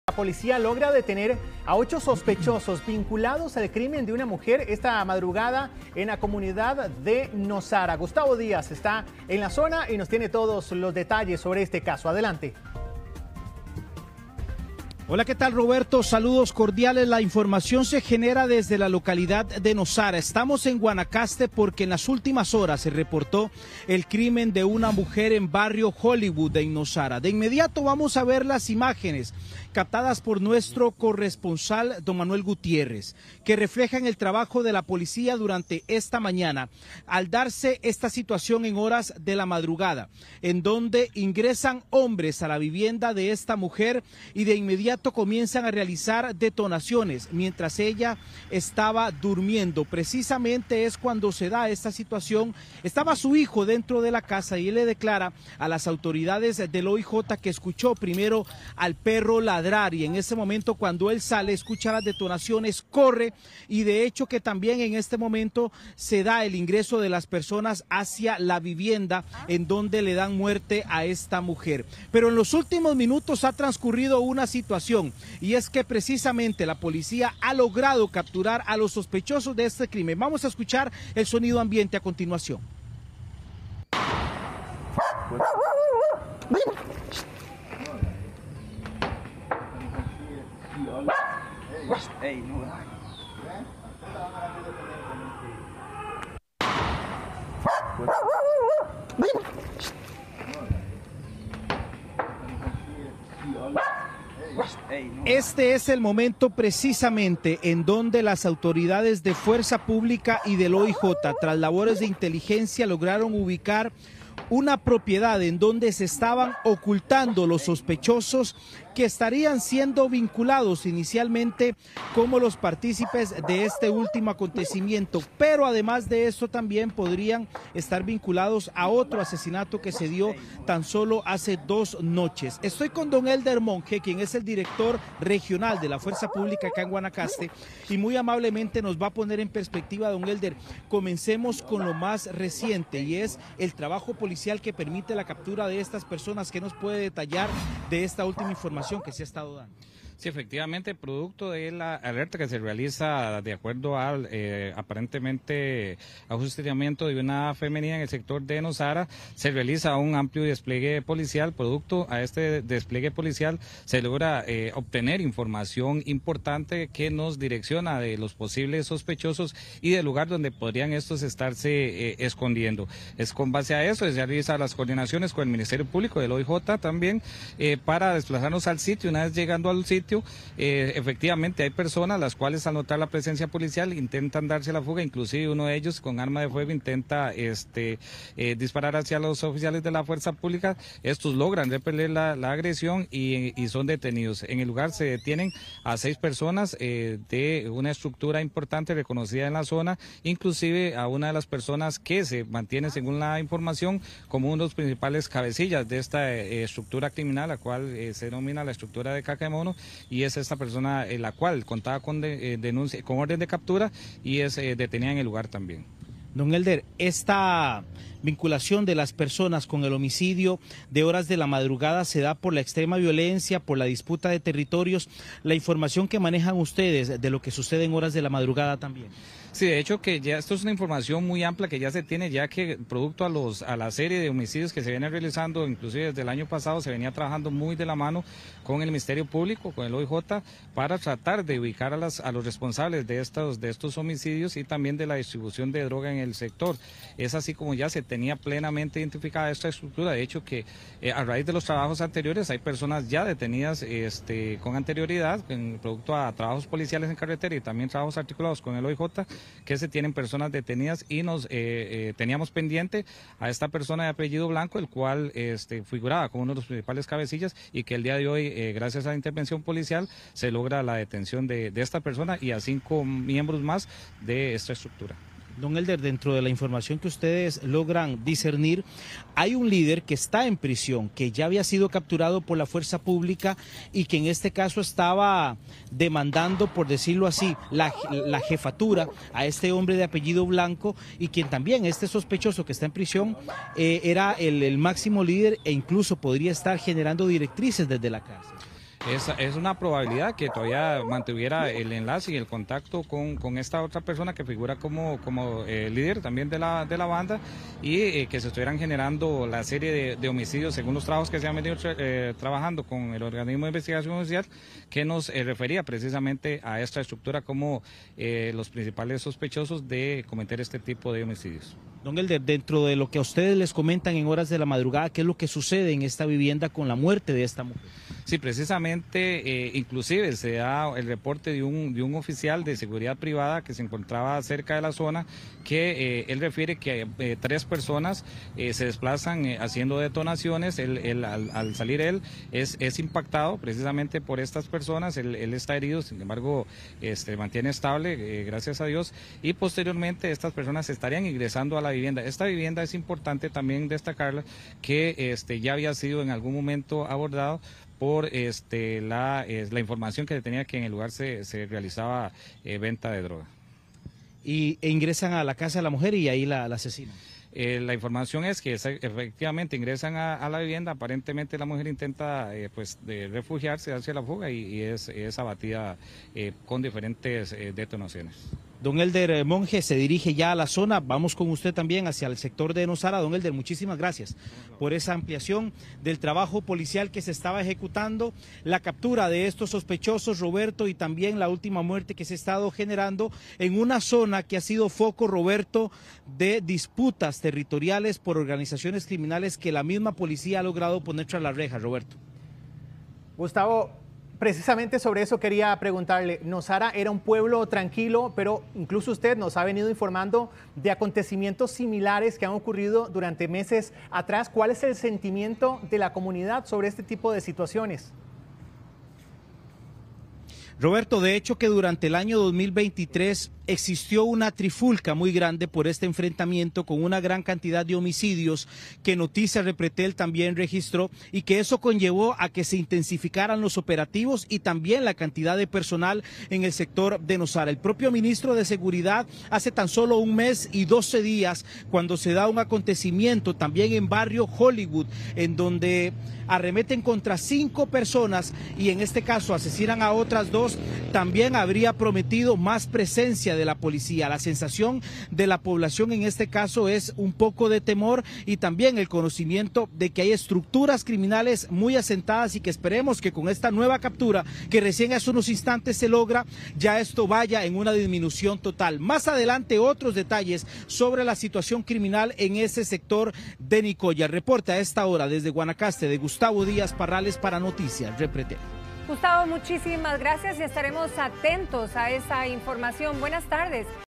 Sicarios asesinan a vecina de Nosara mientras estaba con su hijo en su casa
Autoridades desplegaron operativo y lograron detener a ocho sospechosos.